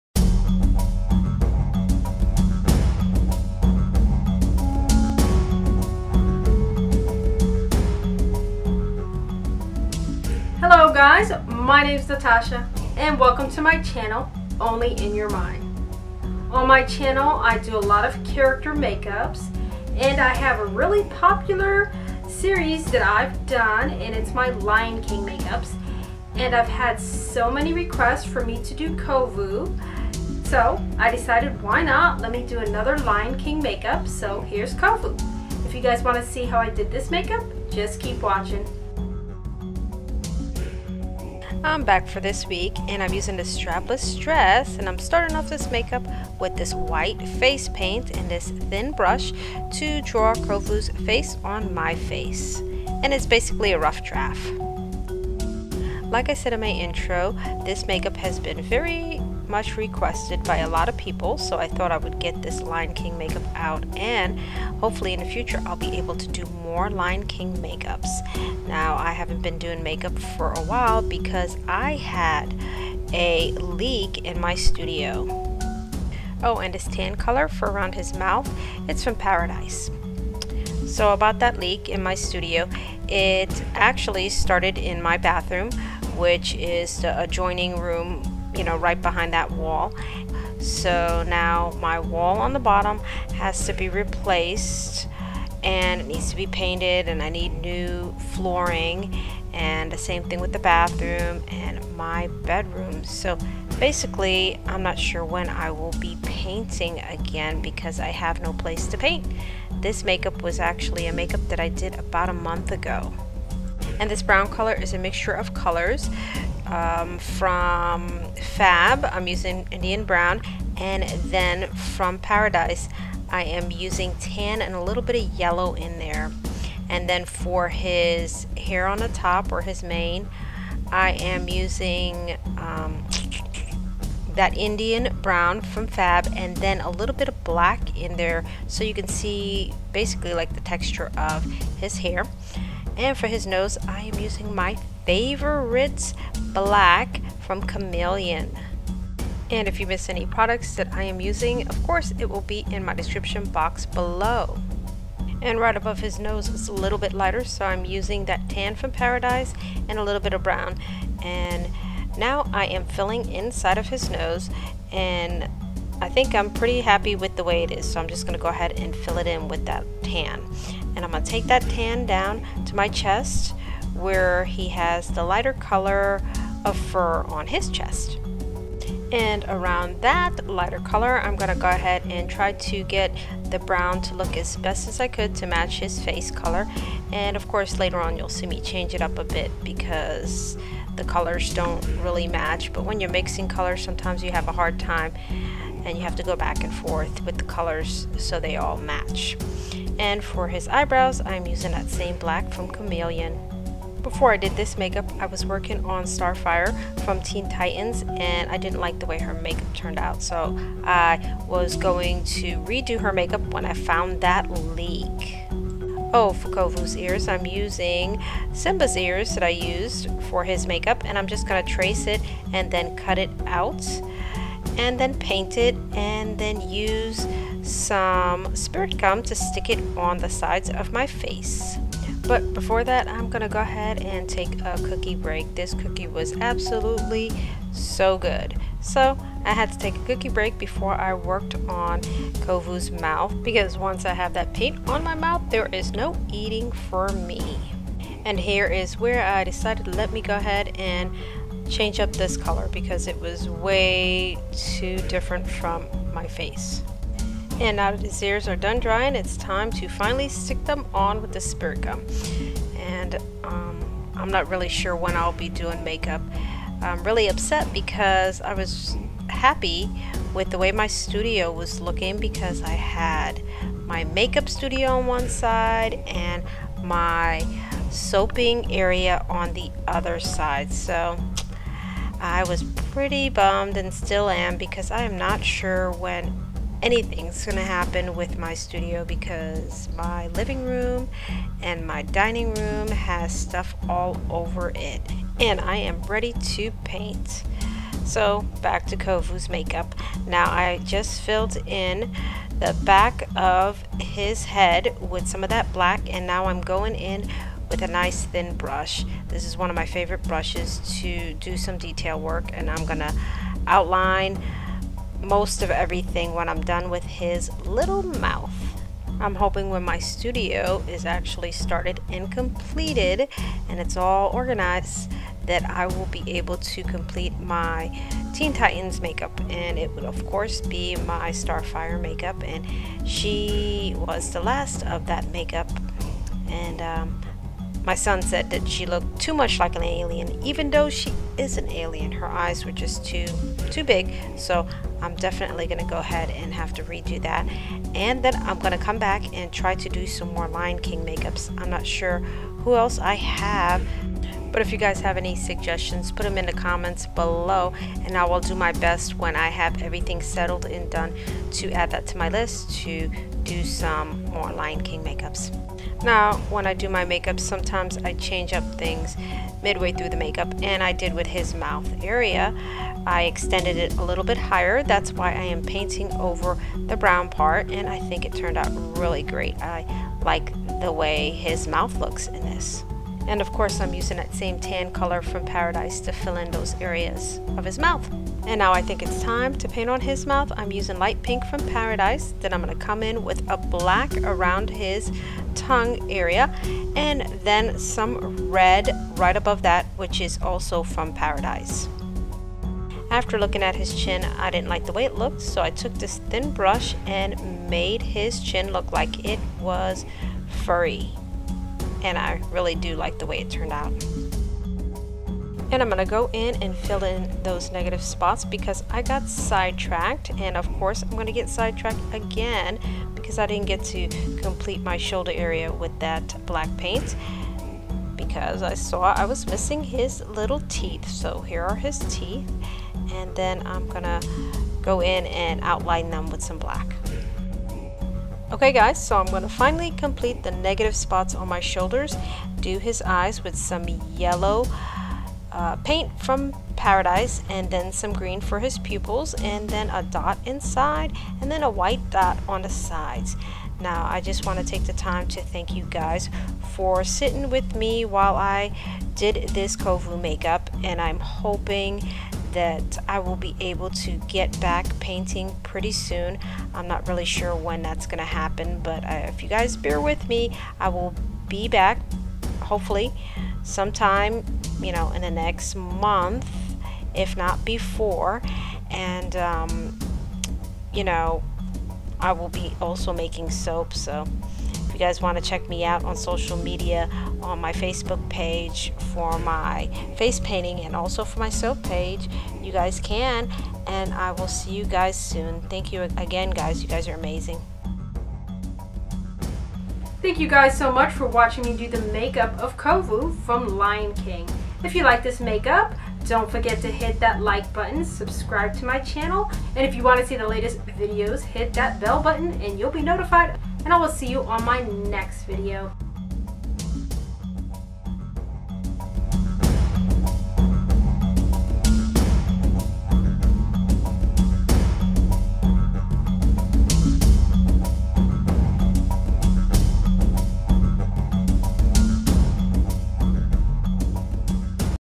The Lion King Kovu Makeup Tutorial
Only In Ur Mind:

Welcome to Only In Ur Mind. I am back with another Lion King makeup.  This time it is Kovu.  I was really happy with the way his makeup turned out.  I also will be doing more makeup’s from Lion King when I get my studio fixed from the water leak.  So I hope you enjoy.

Make-up used
mehron paradise paint: white, light pink, red, yellow, green, and  tan
Cameleon: Black
FAB: Indian brown